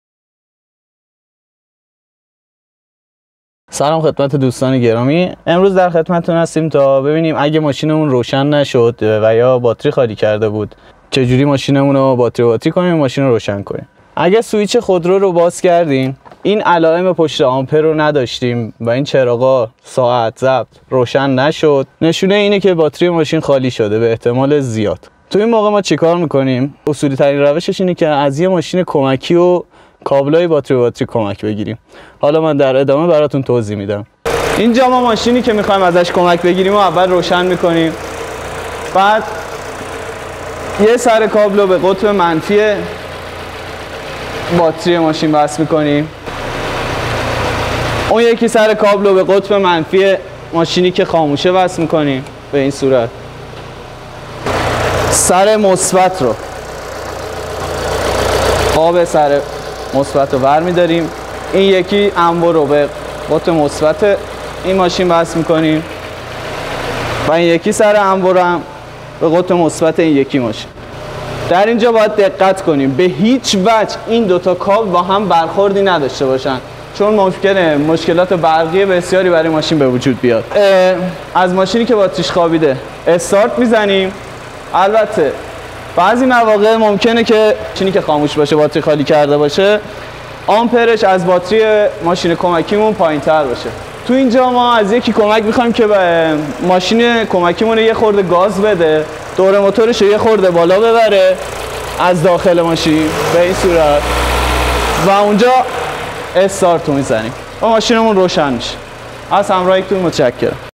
آموزش صحیح باتری به باتری کردن خودرو - همراه مکانیک
در این ویدئو کارشناس همراه مکانیک با آموزش نحوه صحیح باتری به باتری کردن ماشین به شما کمک می‌کند تا درصورت روشن نشدن خودرو و یا خالی شدن باتری، بتوانید خود را به اولین باتری سازی برسانید. جهت مشاهده ویدئوهای بیشتر لطفا به اینستاگرام ما به آدرس روبرو مراجعه بفرمایید: Officialhamrahmechanic - آدرس وبسایت جهت انجام خرید،فروش و کارشناسی خودروی شما